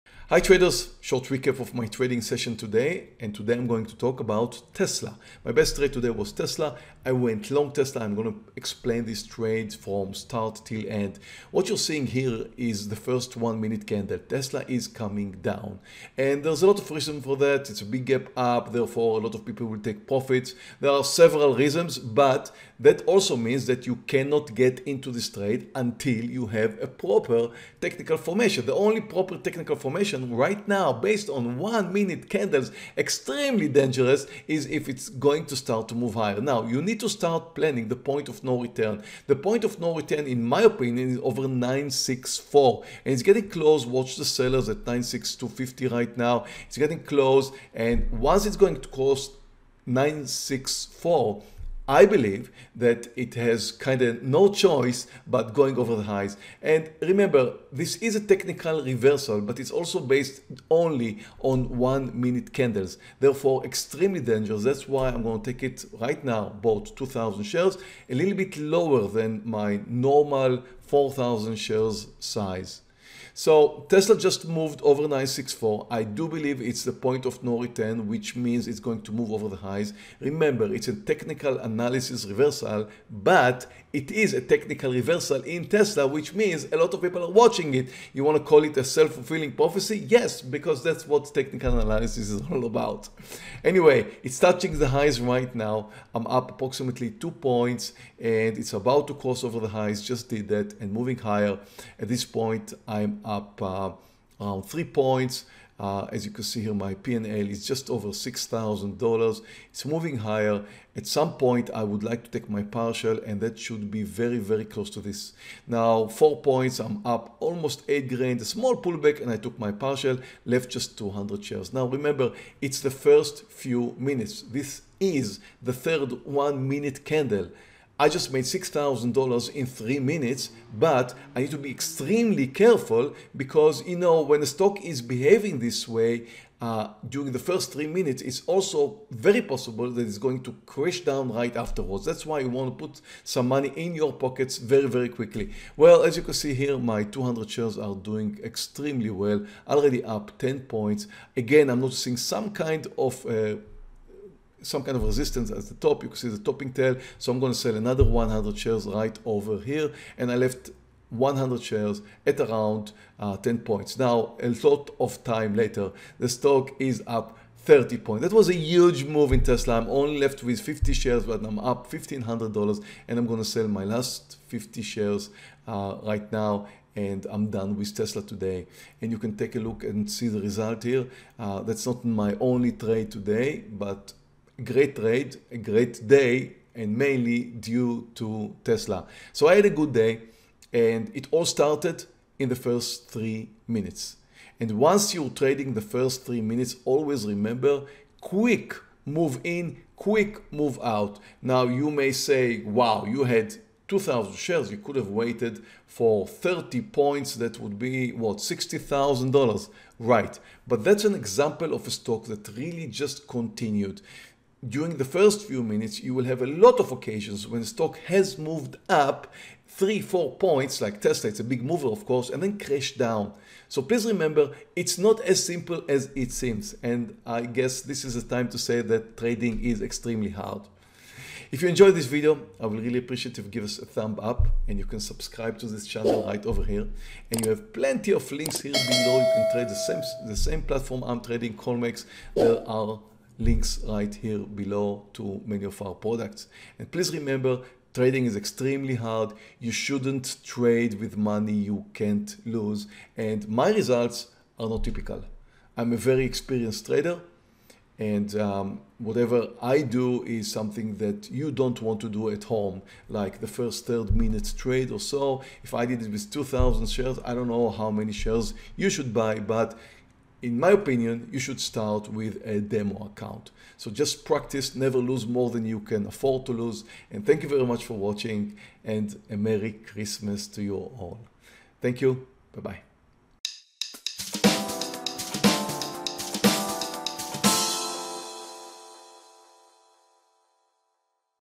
INCREDIBLE 30-POINT TSLA TRADE (LIVE DAY TRADING)
Live Day Trading Stocks - Earning $11,000 trading TSLA, ALLK and BABA. Meir Barak live trading on December 22nd 2021. Incredible 30-point Tesla trade...

Huge discounts! Limited time! Offer ends on Dec. 31st 2021.